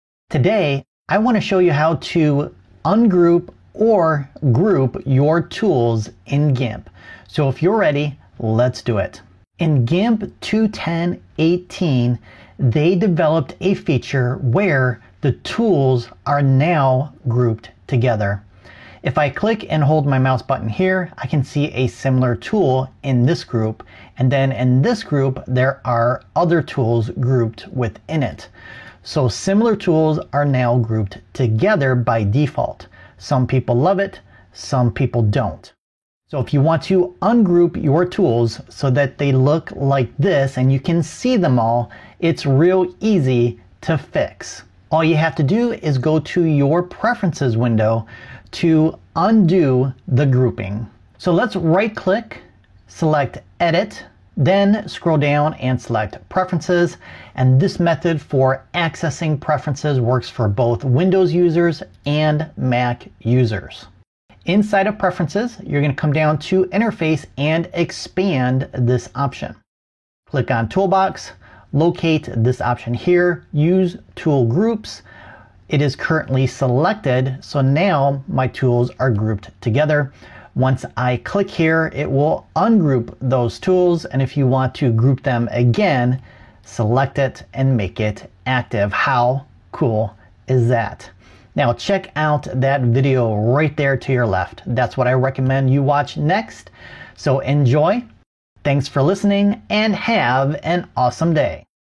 How To Show All Tools in GIMP Or Group Them
In today's video, you'll discover how to show all your tools in GIMP.  Or if you prefer... how to group your tools together.

🅼🅰🆂🆃🅴🆁 🅶🅸🅼🅿 & 🆂🅰🆅🅴 🆄🅿 🆃🅾 80% 🅾🅵🅵!

📸 Worlds BEST Camera

BEST GIMP ALTERNATIVE...